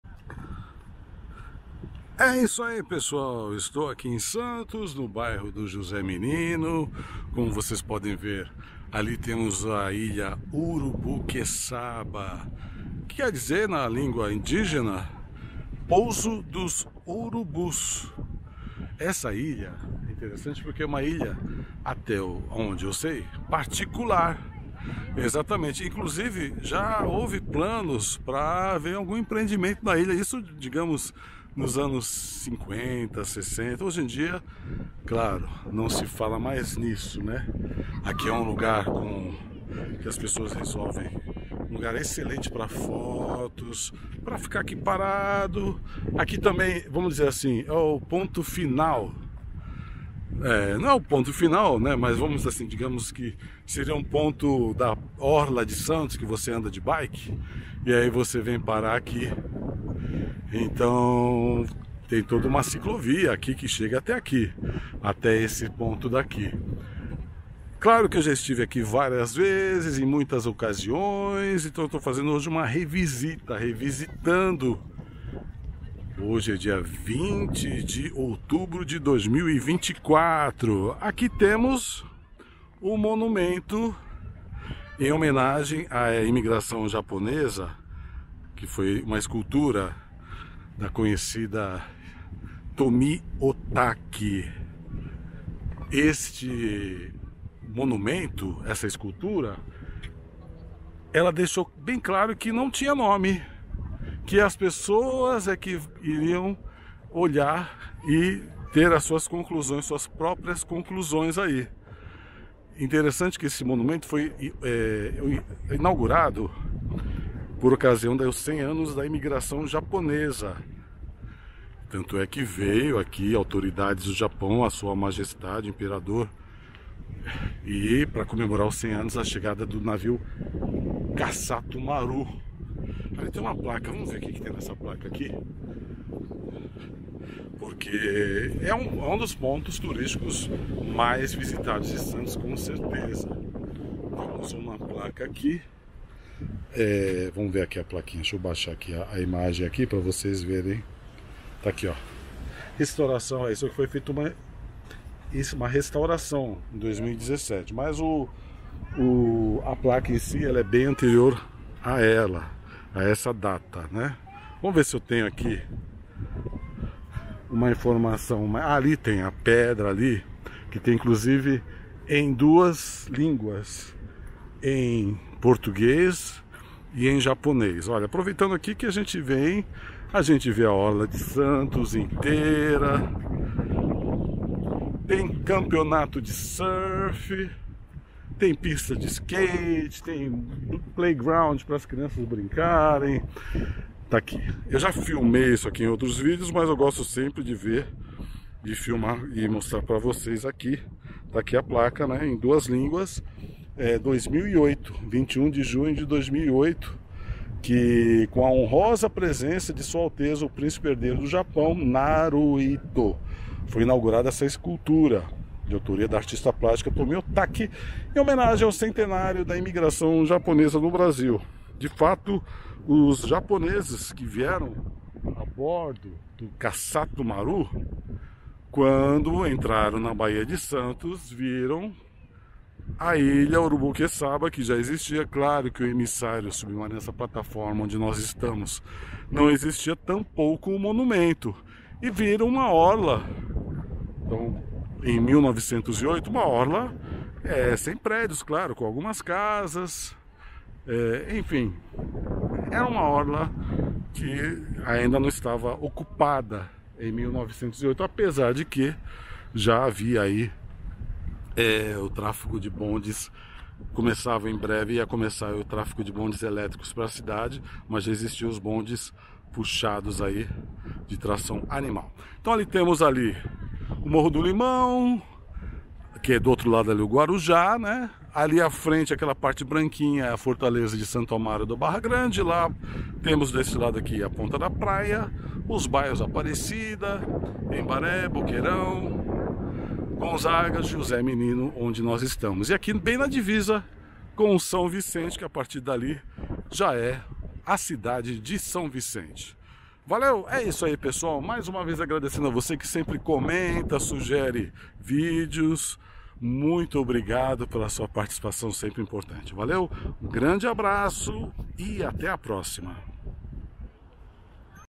No emissário, o monumento e a ilha
Certamente um dos lugares mais visitados em Santos é a plataforma do emissário submarino, no José Menino. Já fui diversas vezes e há vários vídeos aqui no canal.
Pra matar as saudades, está aqui uma visão de um dia de céu nublado, no fim de tarde.
O Monumento em Homenagem aos 100 anos da Imigração Japonesa e ao lado, a Ilha Urubuqueçaba.

Gravado em 21 de outubro de 2024